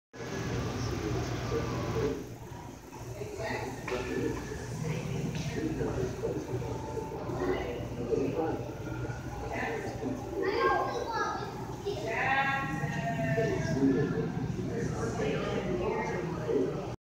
Wood Ducks
Here's wood ducks at the aquarium, a male and a female.